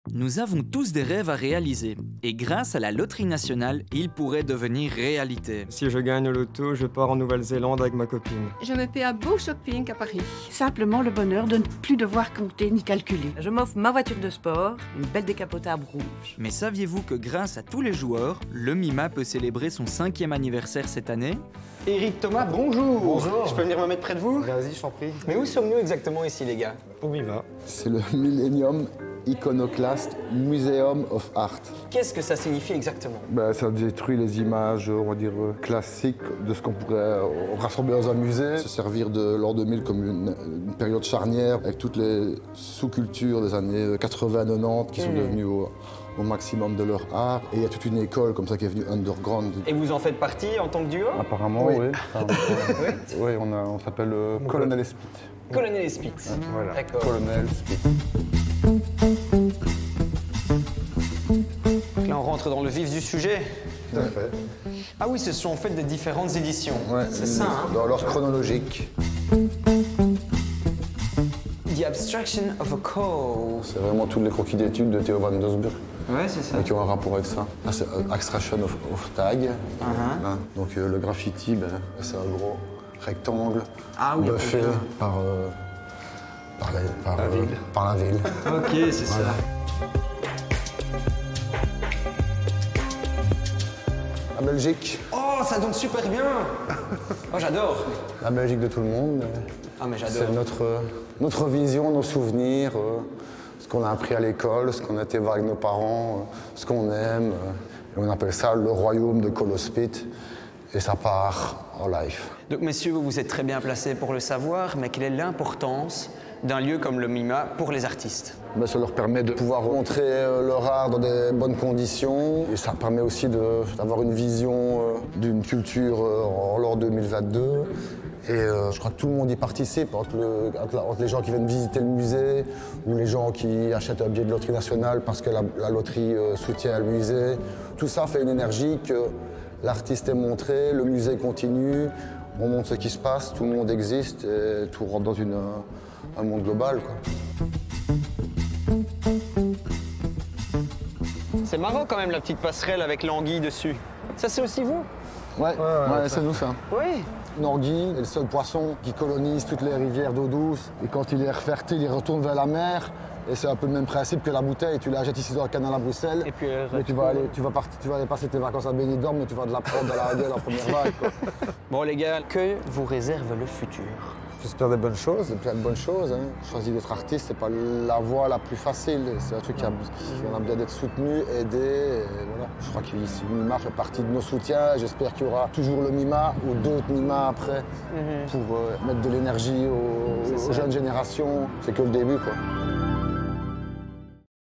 La Loterie Nationale soutient le MIMA
Grâce à ses joueurs, La Loterie Nationale soutient le @mimamuseumbrussels et ses artistes. 🎨 Le Millenium Iconoclast Museum of Art fête ses 5 ans cette année et vous propose de découvrir l’art d’un autre oeil, au centre de Bruxelles.